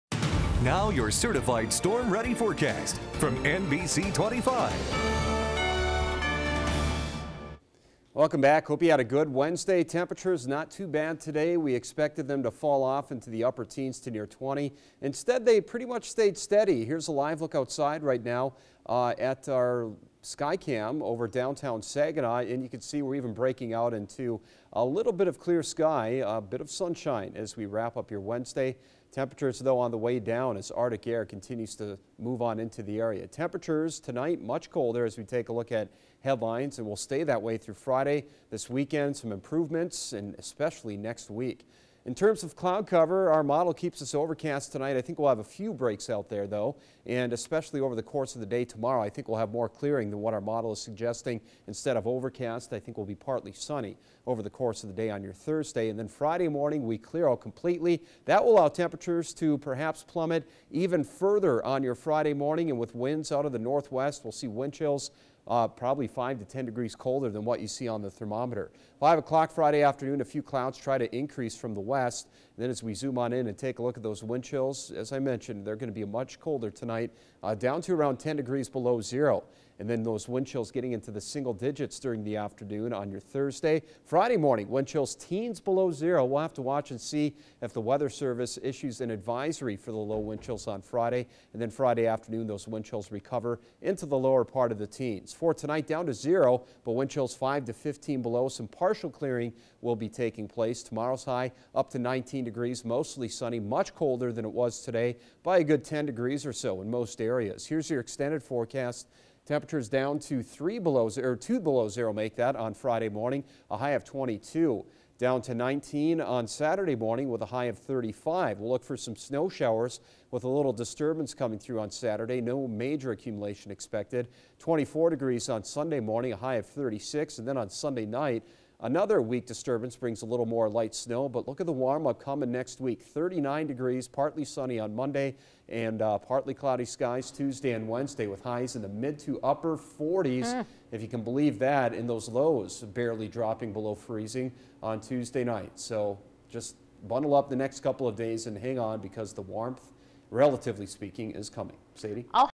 A March cold blast
Temperatures usually go up gradually over the course of March, but we hit a speed bump tomorrow and Friday.